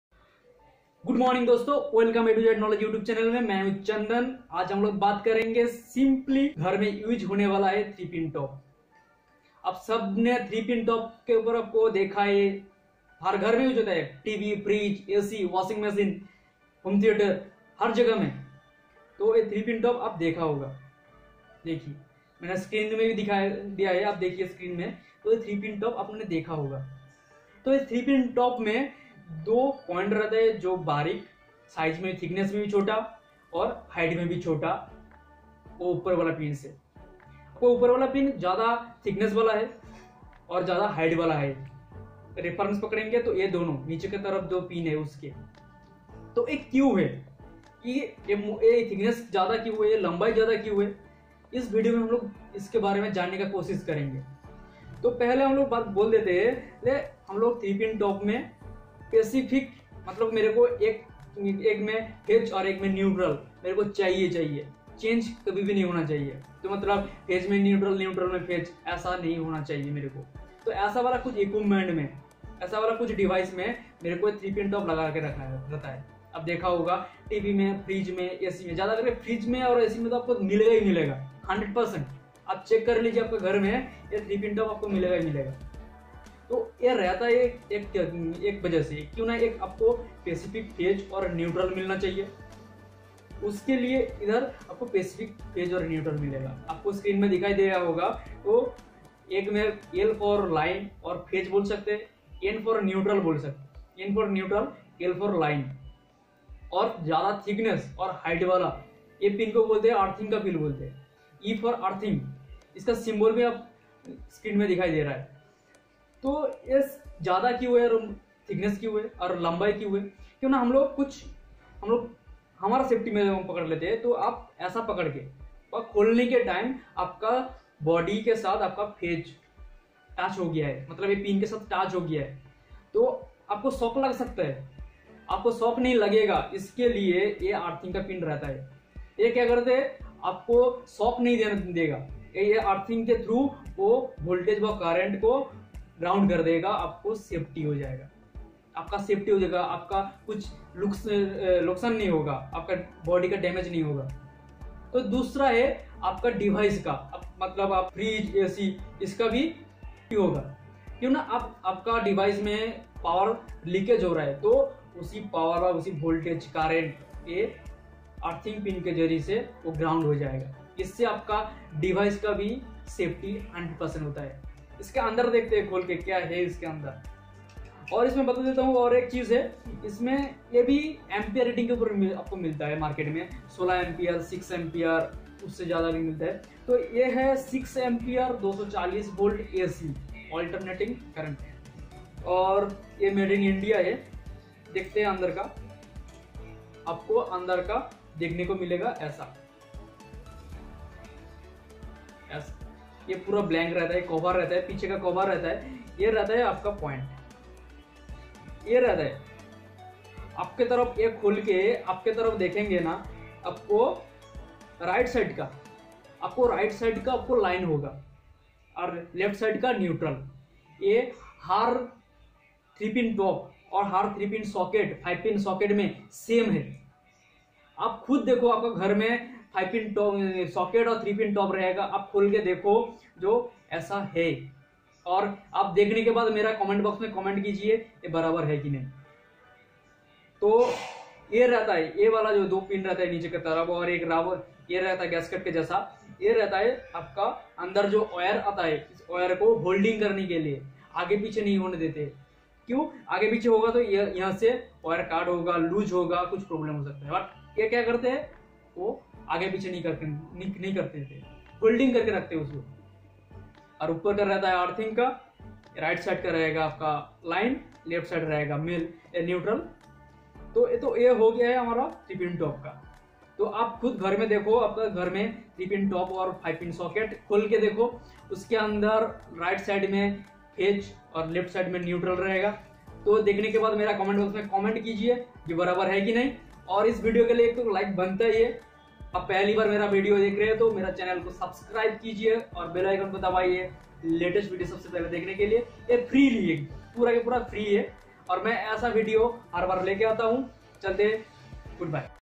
why 3 pin plug is used
# 3pin top's third pin bigger & larger because human & device safety. And we don't wrong connection. And want to specific phase & neutral & Earth.

                            " A TO Z KNOWLEDGE"

Any questions to contact by - Comment